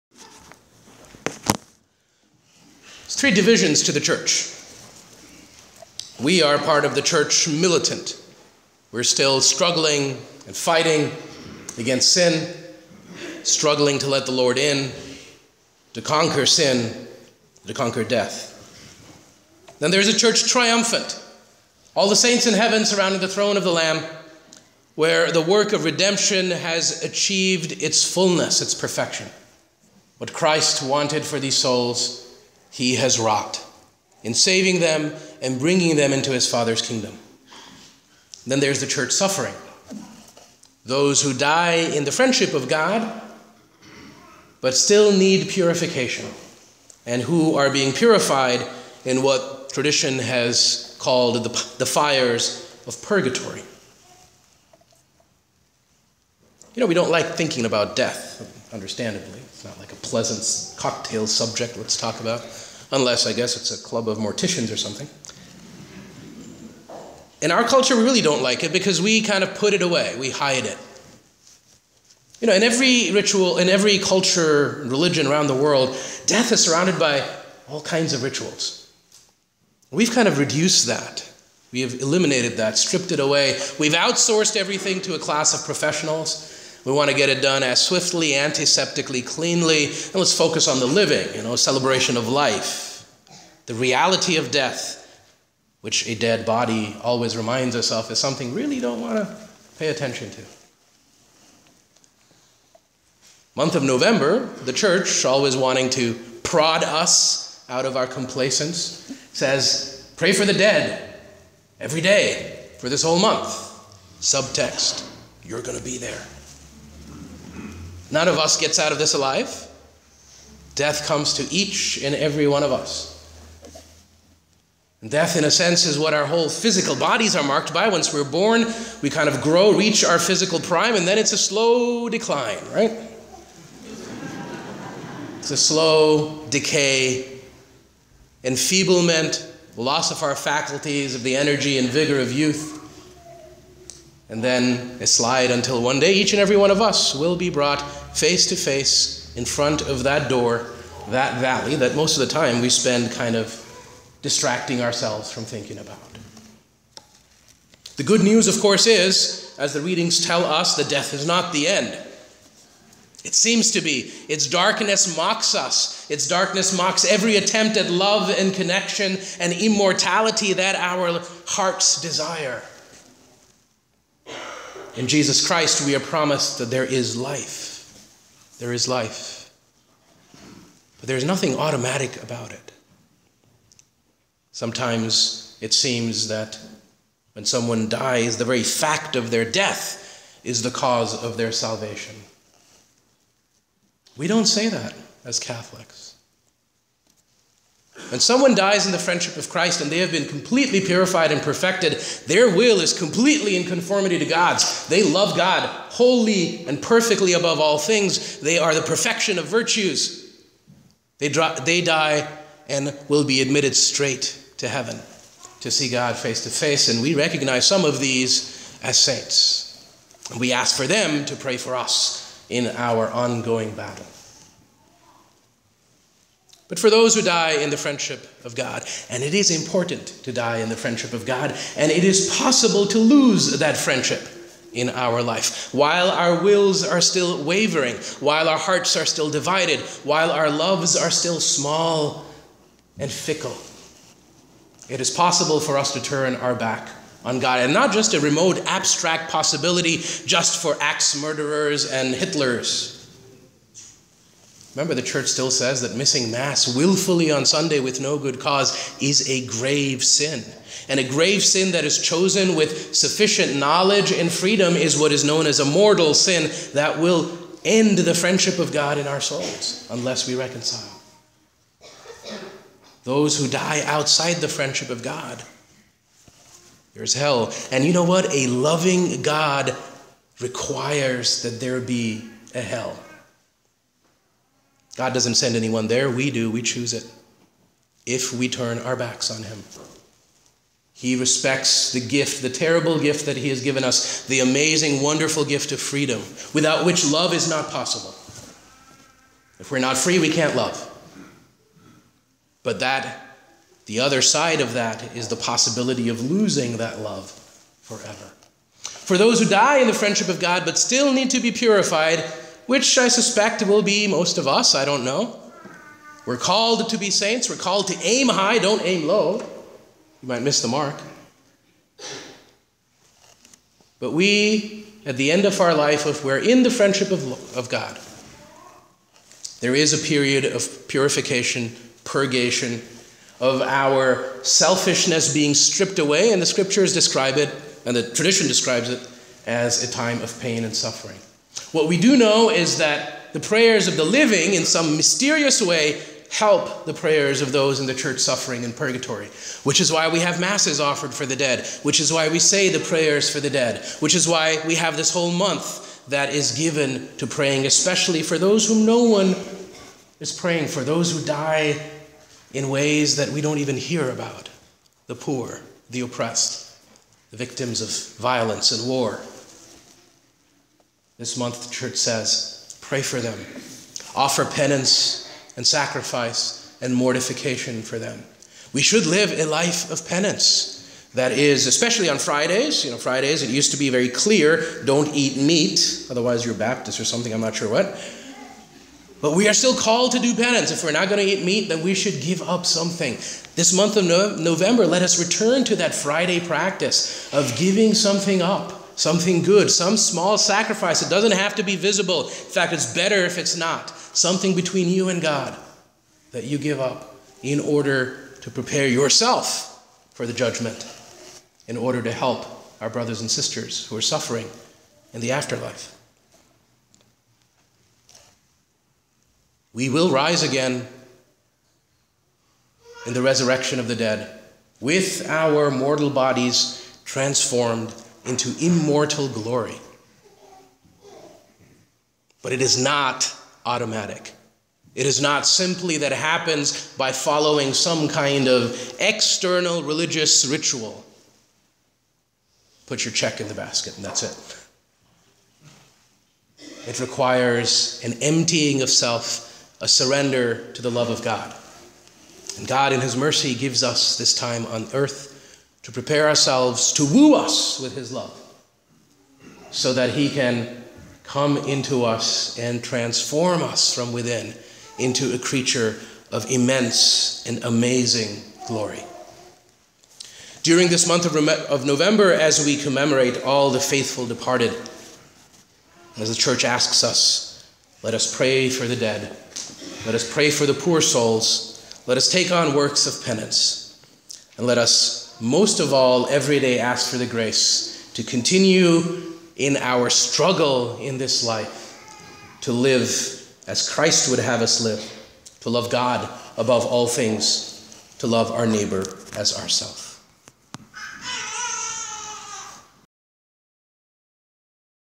All Souls Day 2014
Homily for All Souls Day, St. Joseph Parish, Athens GA, November 2, 2014.